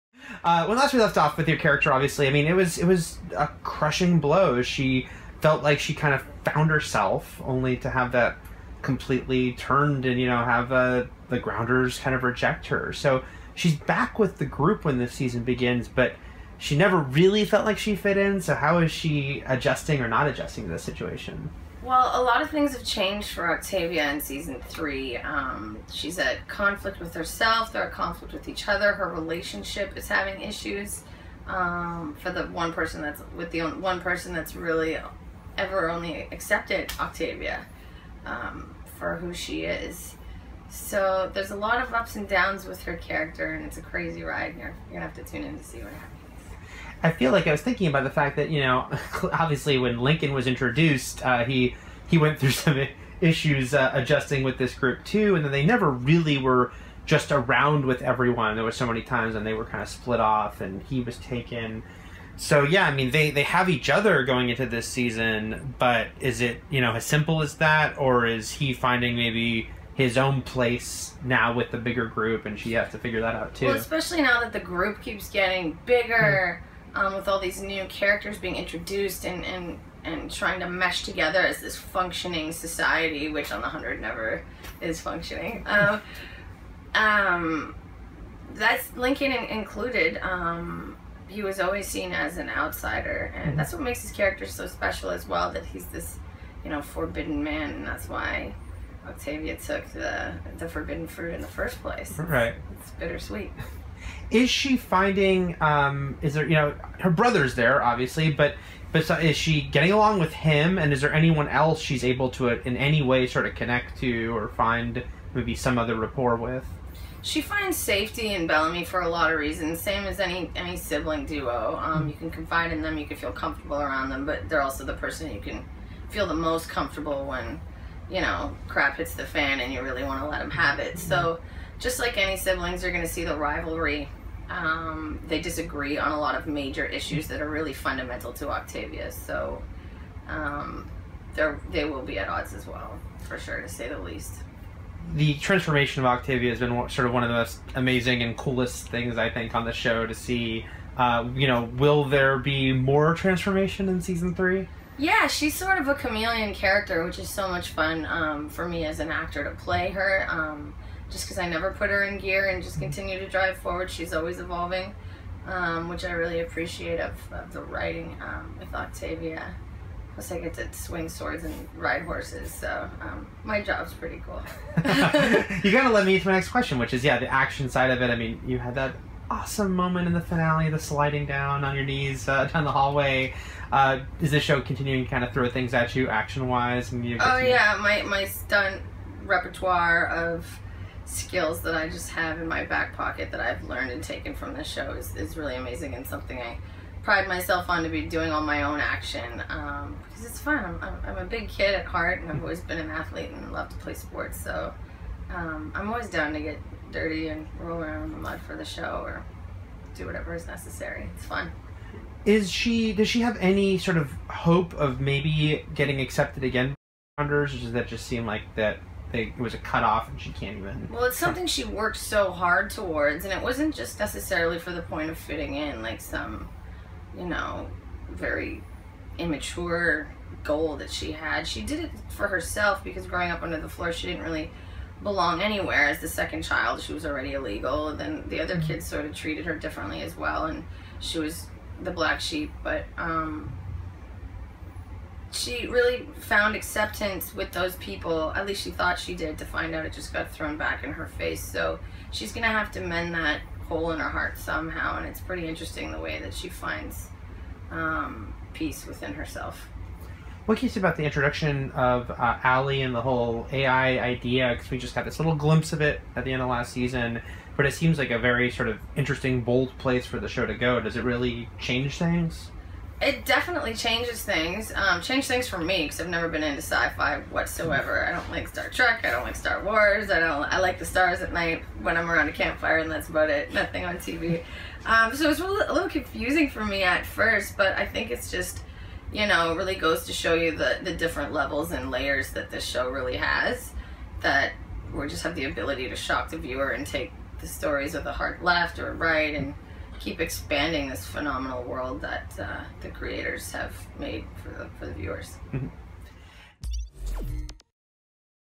The 100: Octavia's Struggles with Lincoln and Bellamy in Season 3 - Marie Avgeropoulos Interview
On the set of The 100: Season 3, Marie Avgeropoulos discusses what's next for Octavia Blake after being rejected by the Grounders.

Follow IGN for more!